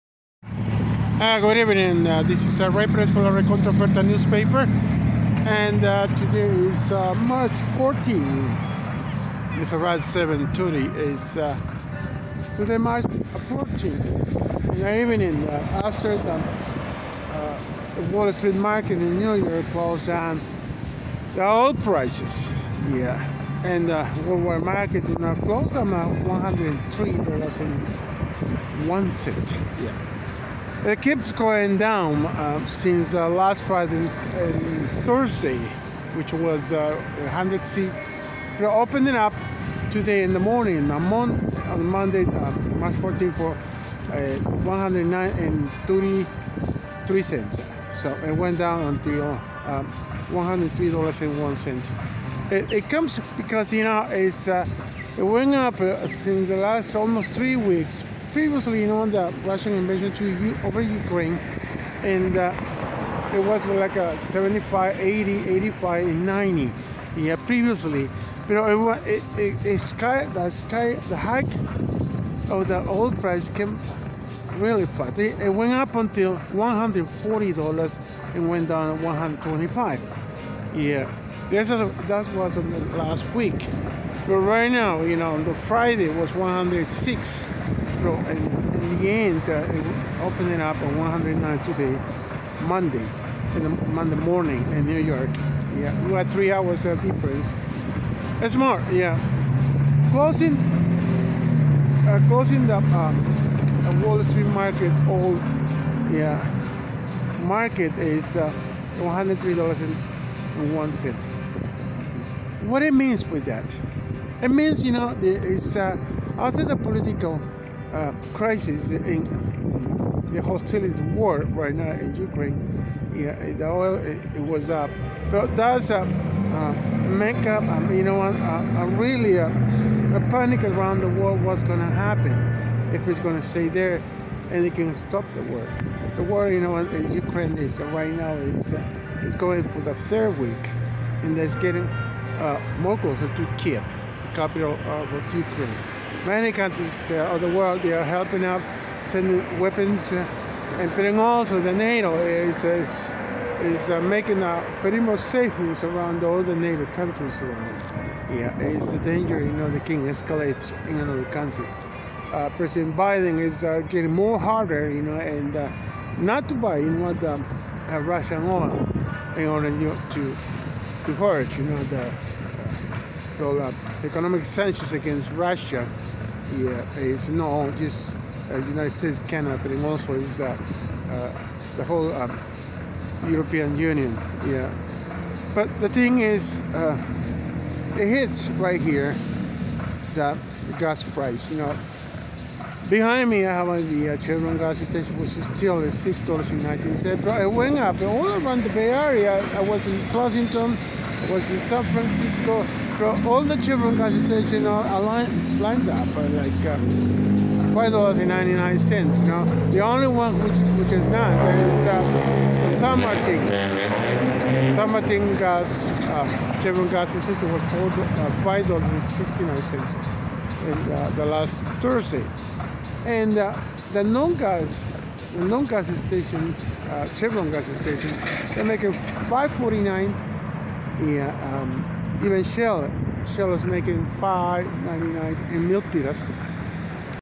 U.S., Oil Price goes down $103.01 per barrel on Monday, March, 14, 2022.
U.S., Oil Price goes down $103.01 per barrel on Monday, March, 14, 2022. and prones to keep going down.

Por technical reasons this video Economic report was lost and I apology for the inconvenience.